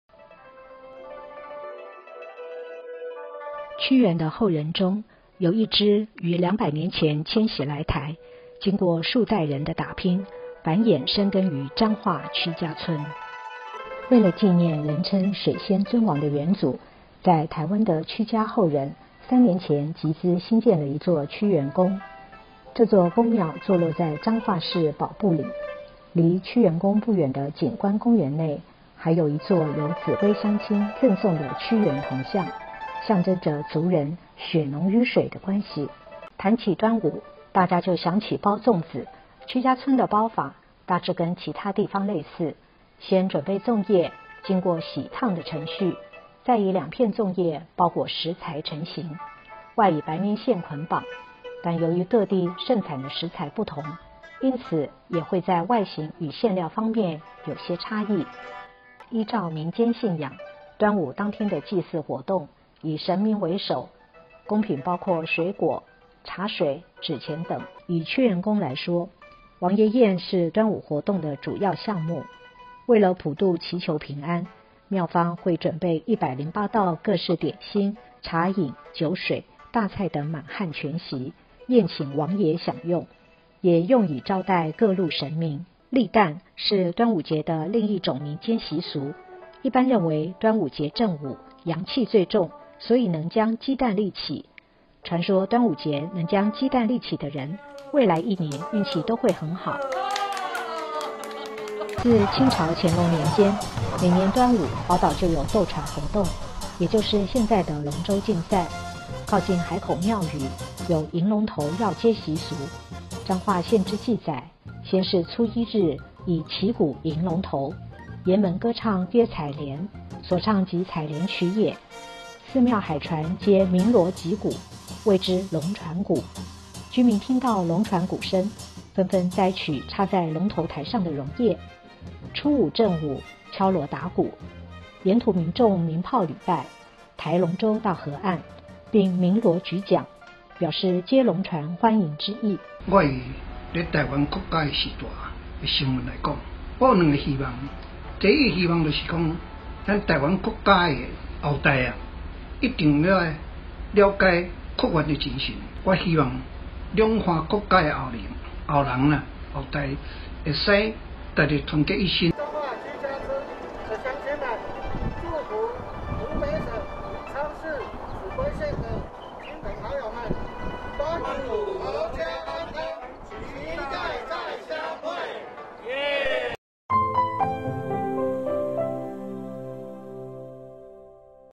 台湾屈氏后裔“打call”湖北秭归“云”庆端午
临近端午，来自台湾彰化屈家村的屈氏后裔拍摄短片《屈家追远庆端阳》，为屈原故里湖北秭归“打call”，视频不仅展示了彰化端午习俗，在“云端”庆端午，也“隔空”祝福秭归同胞“端午阖家安康，期待早日相聚”。
        端午节是两岸民众都十分重视的节日，受疫情影响，在台屈氏后裔无法来秭归参加文化节活动，他们选择留在家中与亲友一起包粽子、话家常。视频中，一位台湾屈氏长老传达了自己的期盼：“希望台湾的屈氏后裔要了解屈原的精神，两岸屈氏后裔能够团结一心。”